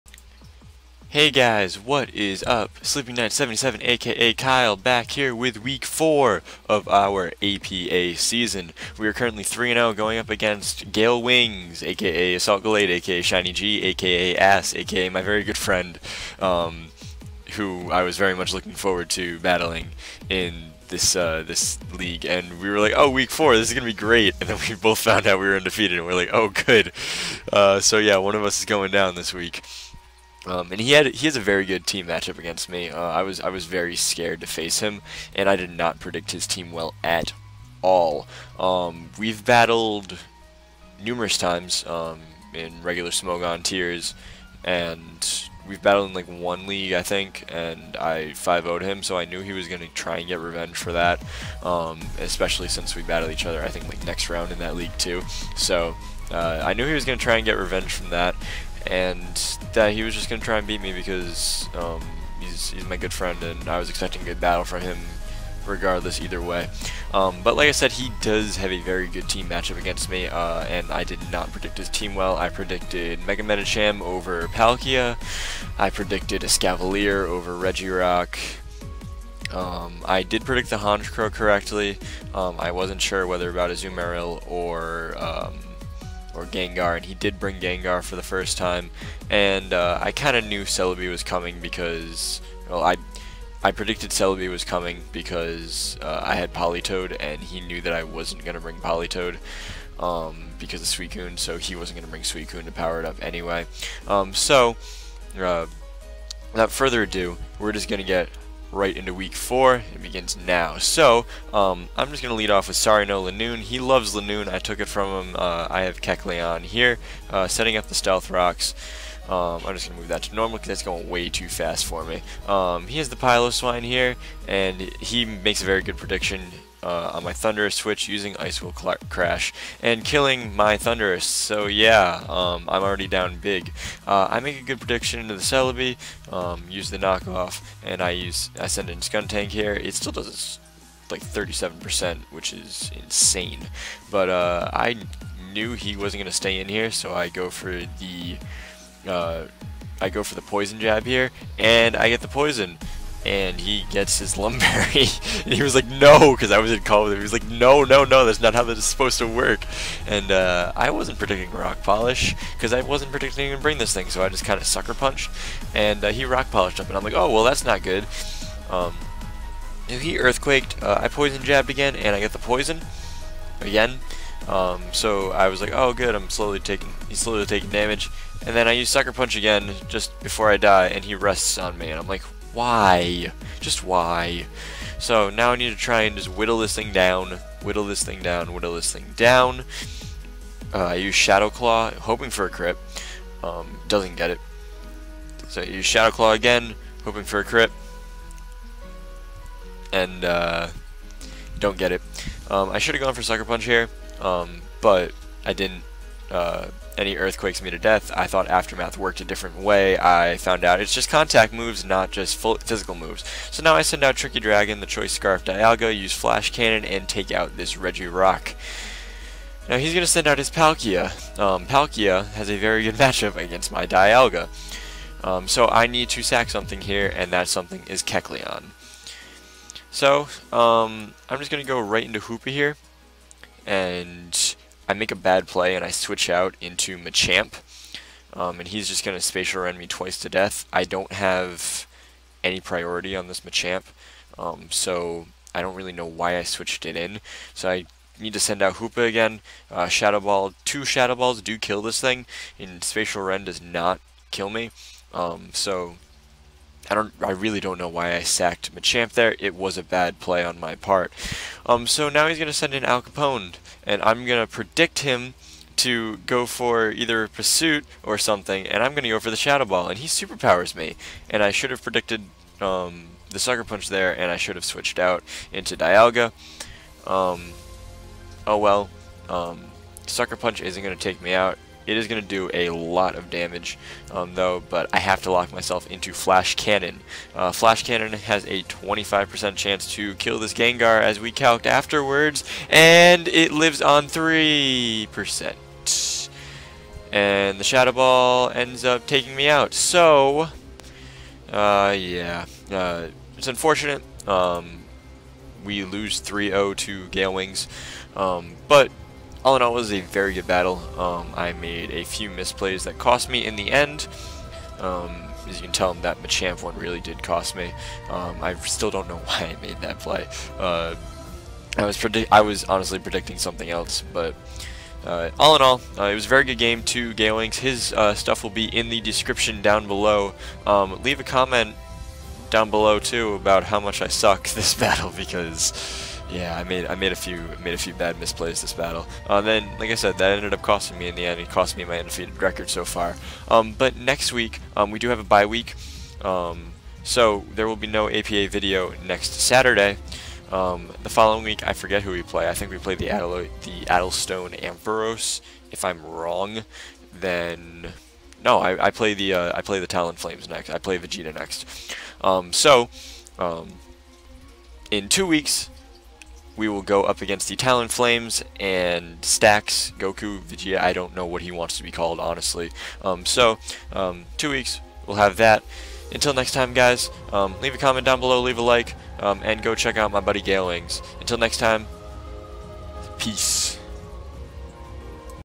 APA Week 4 PRO (3-0) vs. Southwest Sandiles (3-0)
Hey guys! Kyle here, and today we play GaleWings and the Southwest Sandiles looking to keep our undfeated record in tact. Will we succeed?